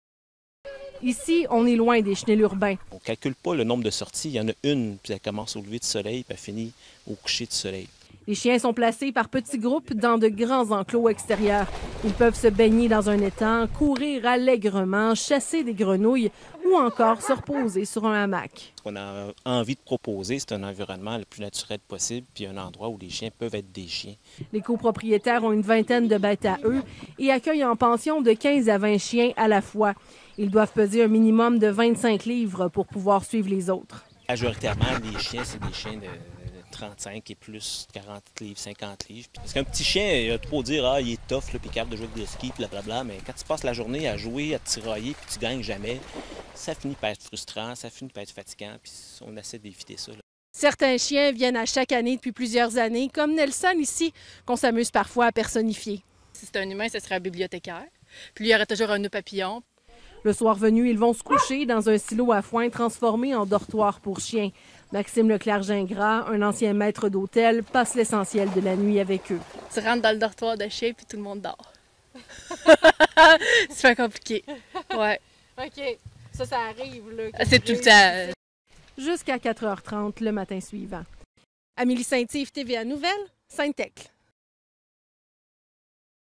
Un camp de vacances pour chiens
Un ranch canin situé à Sainte-Thècle, en Mauricie, accueille chaque été des dizaines de chiens dans sa formule «camp de vacances».

Au Ranch canin Manitou, on est loin des chenils urbains.

«On ne calcule pas le nombre de sorties. Il y en a une. Elle commence au lever du soleil et se termine au coucher du soleil», a expliqué vendredi le copropriétaire Maxime Leclerc-Gingras, en entrevue avec TVA Nouvelles.

Les chiens sont placés par petits groupes dans de grands enclos extérieurs. Ils peuvent se baigner dans un étang, courir allègrement, chasser des grenouilles... ou se reposer sur un hamac.

Environnement naturel
«Ce qu’on a envie de proposer, c’est un environnement le plus naturel possible, et un endroit où les chiens peuvent être des chiens», a poursuivi M. Leclerc-Gingras.

Les copropriétaires ont une vingtaine de bêtes à eux, et accueillent en pension de 15 à 20 chiens à la fois. Ils doivent peser un minimum de 25 livres, pour pouvoir suivre les autres.

«Majoritairement, [on accueille des chiens de] 30, 40, 50 livres. Un petit chien, tu as beau dire qu’il est "tough" et qu’il est capable de jouer avec des Huskies, mais quand tu passes la journée à jouer, à te tirailler et que tu ne gagnes jamais, ça finit par être frustrant et c’est ce qu’on veut éviter», a-t-il indiqué.

Le soir venu, ils vont se coucher dans un ancien silo à foin transformé en dortoir. Maxime Leclerc-Gingras, un ancien maître d’hôtel, passe l’essentiel de la nuit avec eux.

«Tu rentres dans le dortoir de chiens et tout le monde dort», a dit en riant Anne-Marie Charest, également copropriétaire.

Du moins... jusqu'à 4 h 30 le matin suivant!